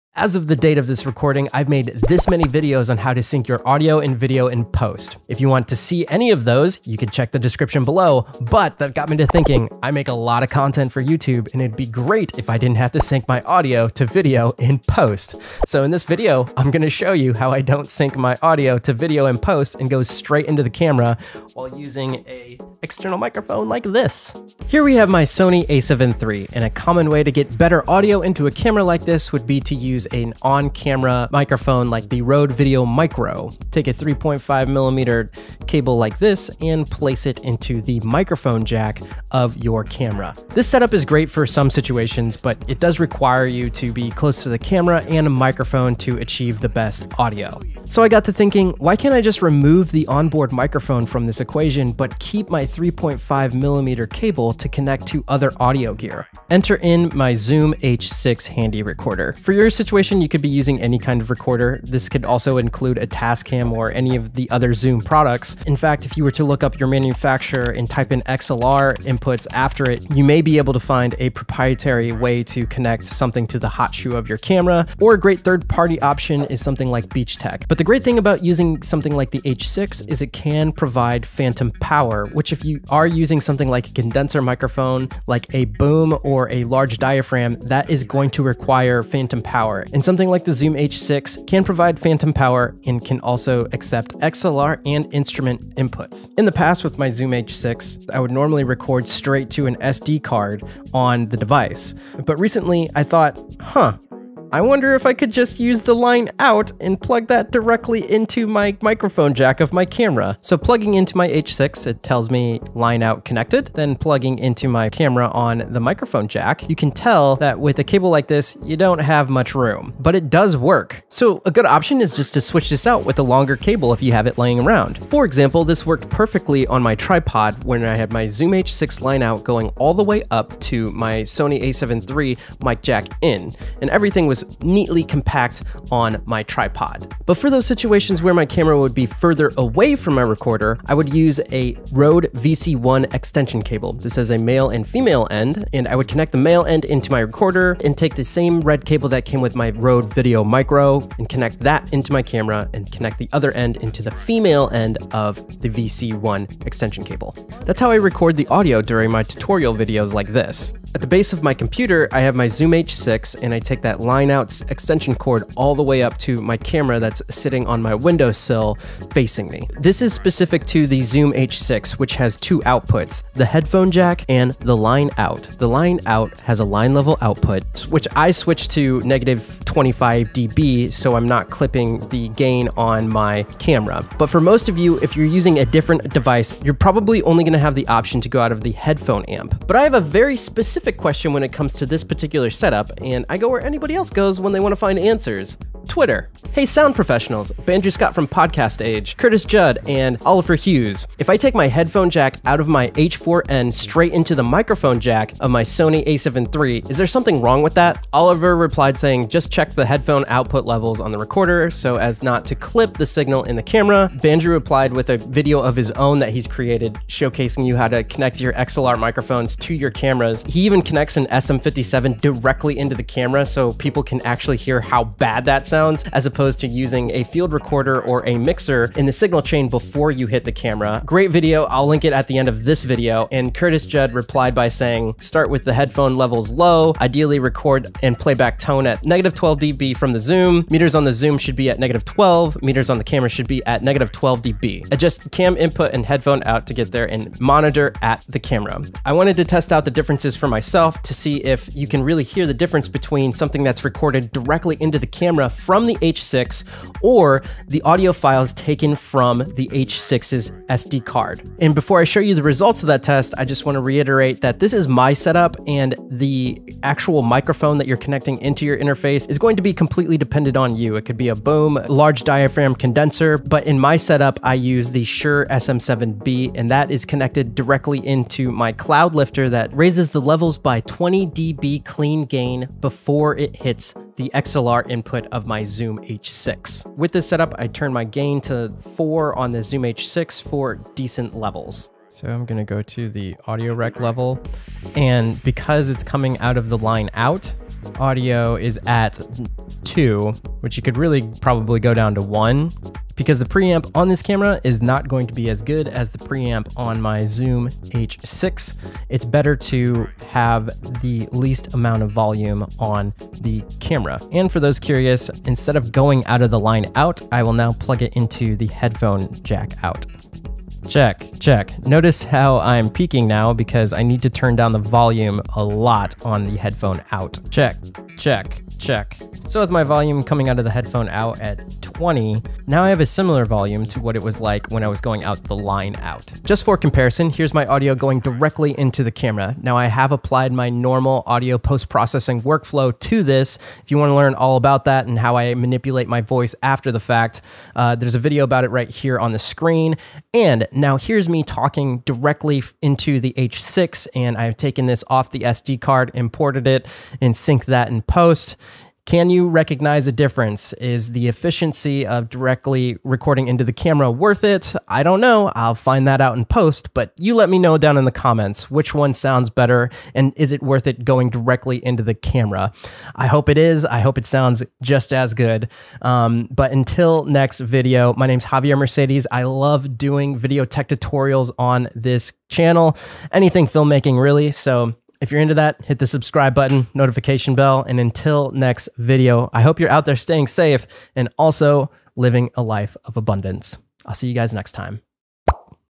How to setup video gear so you don't have to sync audio in post
A tutorial showing you how to setup video gear so you don't have to sync audio in post. I am connecting audio gear to a mirrorless camera using an XLR microphone to connect directly into my camera with the help of the Zoom h6 field recorder line out (or headphone out) plugging into my microphone in jack of my camera. I also test the difference in sound from recording directly to the recorder's SD card vs the direct line audio to the camera. Can you tell the difference in the quality of how it sounds?

Gear Featured:
Rode Video Micro
Sony a7iii
My Mic Shure SM7B
My recorder Zoom h6
Rode VC 1 Extension Cord
3.5mm Aux Cord
Cloud Lifter

My Main Sony lens 50mm f/1.8
My favorite piece of gear DinKum Systems Action Pod Pro